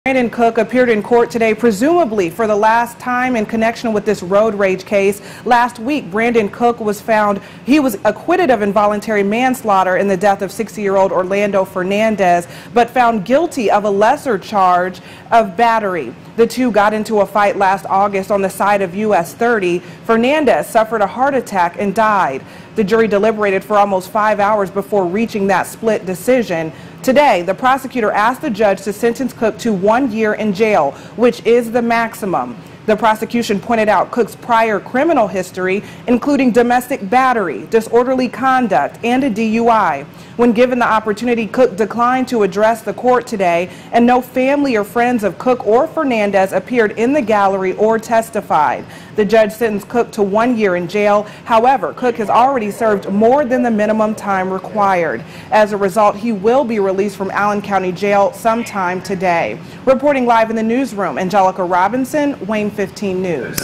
Man in deadly road rage fight to be released from jail
WANE 15 Angelica Robinson reports.